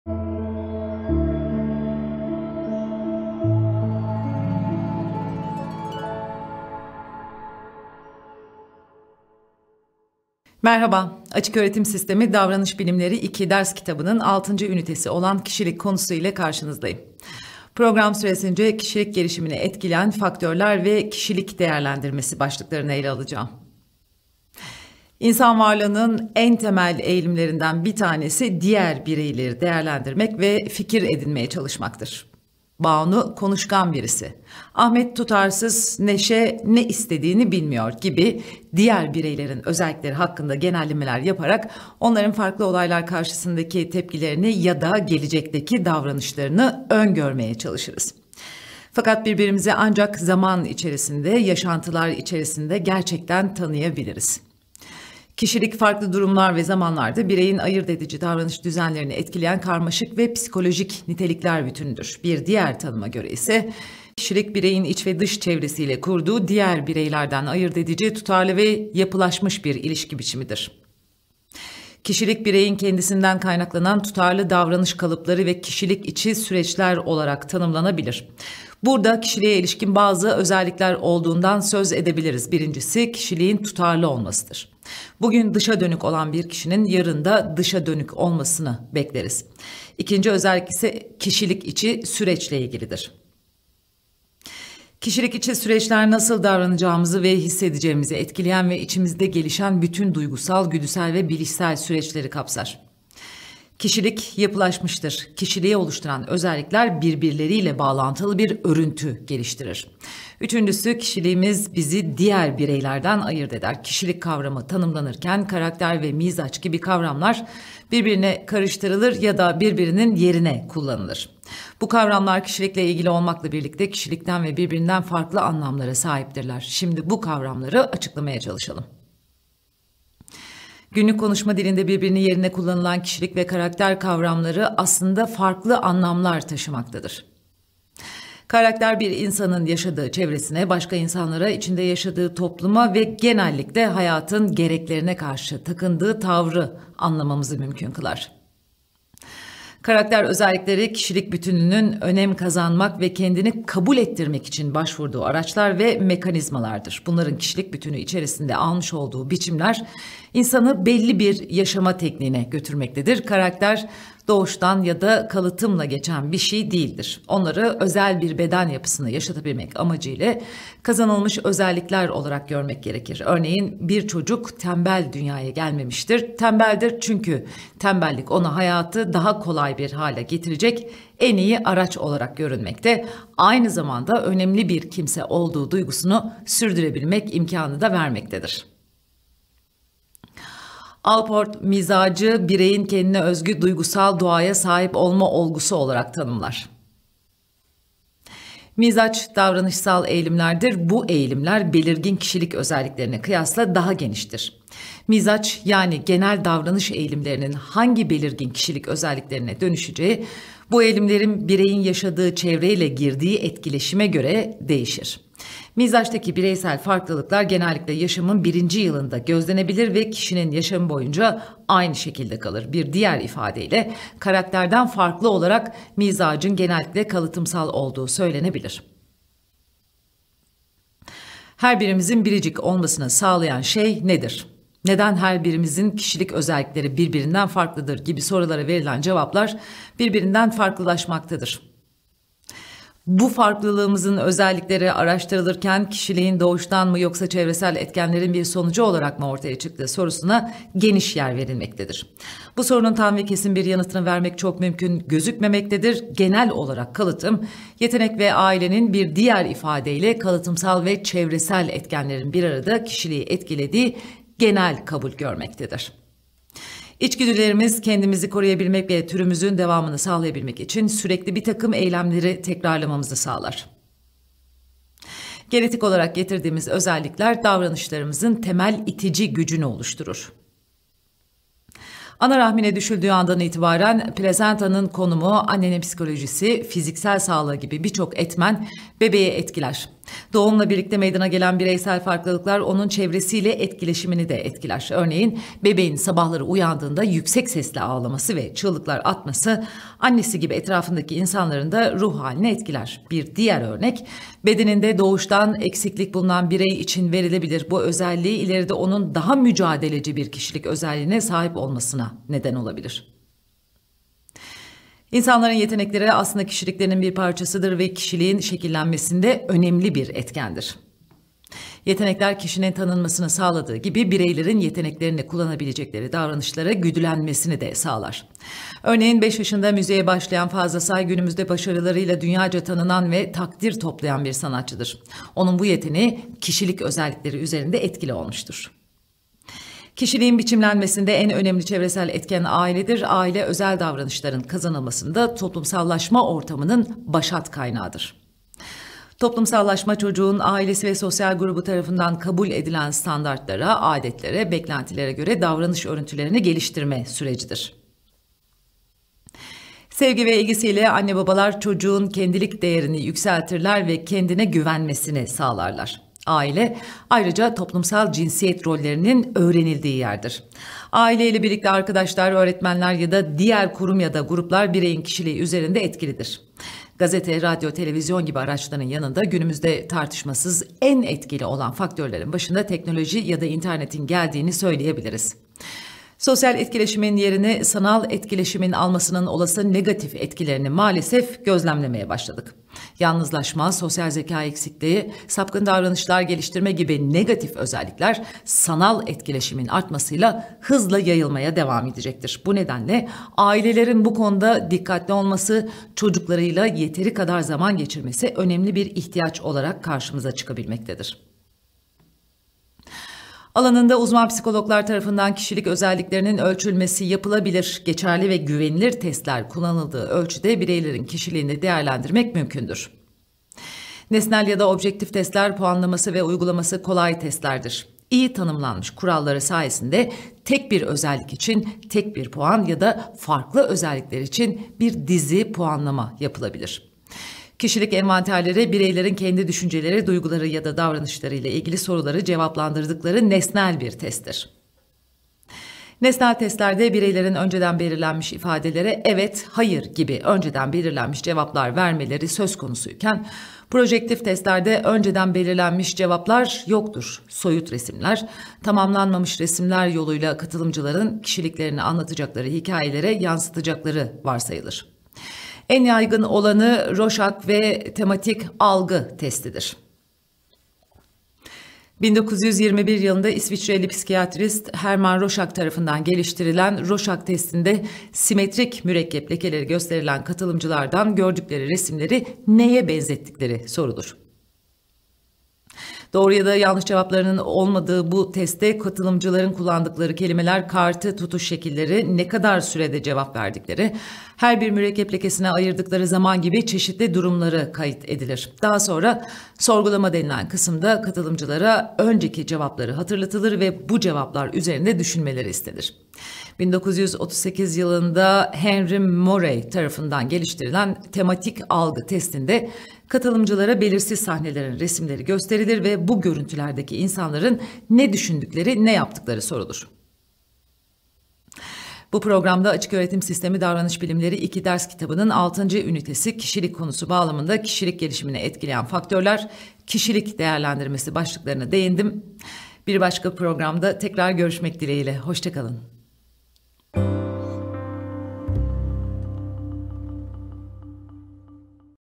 DAVRANIŞ BİLİMLERİ II - Ünite 6 Konu Anlatımı 1
Kişiliği tanımlayabileceksiniz.
Kişilik gelişimini etkileyen faktörleri açıklayabileceksiniz.
Kişilik kuramlarının kişilik gelişime ilişkin yaklaşımlarını analiz edebileceksiniz.
Kişiliğin nasıl değerlendirildiğini açıklayabileceksiniz.